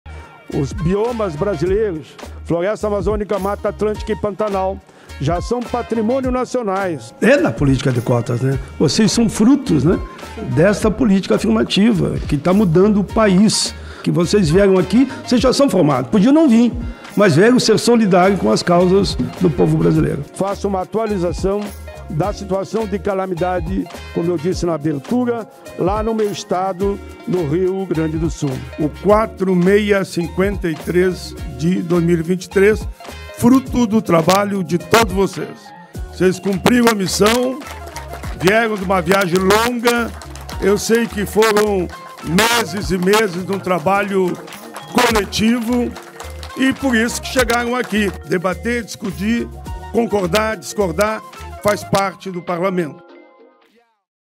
Sextouuuu
Olá, meus amigos e minhas amigas!!! Semana produtiva em Brasília. Realizamos audiências públicas na CDH para debater a transição energética no RS e a Construção do Estatuto do Trabalho. Tivemos reunião com o presidente do Senado, Rodrigo Pacheco e representantes dos movimentos negros, sobre a votação do projeto que avalia a Lei de Cotas. Recebi várias agendas em meu gabinete e concedi entrevista ao Fantástico, sobre os 20 anos do Estatuto da Pessoa Idosa, de nossa autoria. Será transmitido no próximo domingo. Acompanhe! Bom fim de semana!